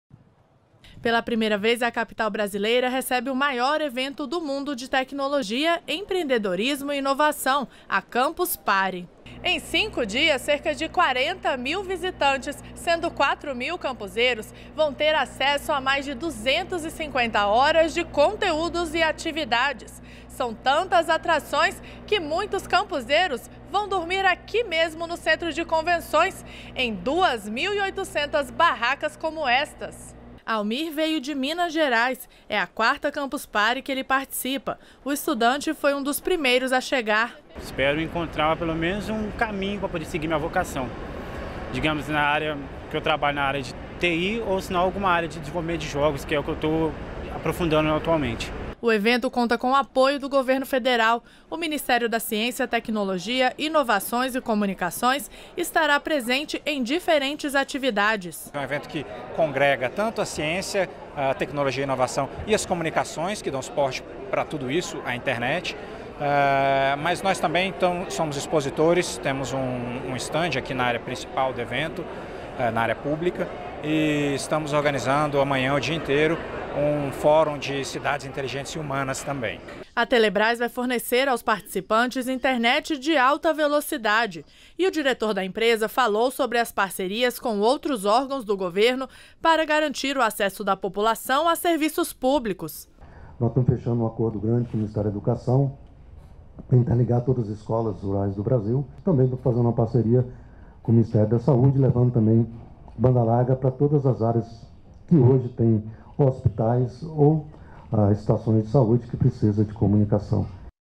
Brasília recebe a Campus Party pela primeira vez
REPÓRTER NBR - 14.06.17: Entre os dias 14 e 18 de junho acontece a Campus Party, o maior evento de tecnologia e empreendedorismo do mundo. Pela primeira vez, o evento ocorre em Brasília. Cerca de 40 mil visitantes terão acesso a 250 horas de conteúdo e atividades.